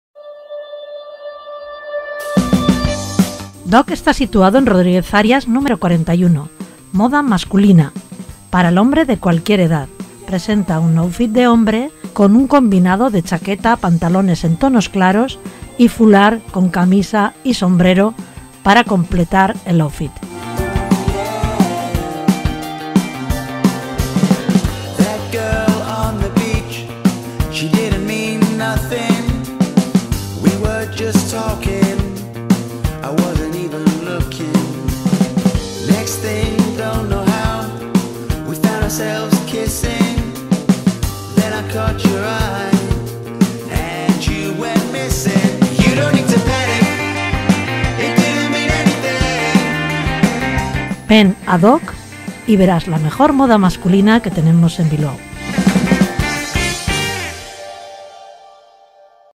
DOCK - Look del Verano BilbaoCentro 2021 - Ganador categoría masculina
El look del comercio Dock resultó ganador en la categoría masculina de "El Mejor Look del Verano 2021" organizado por BilbaoCentro.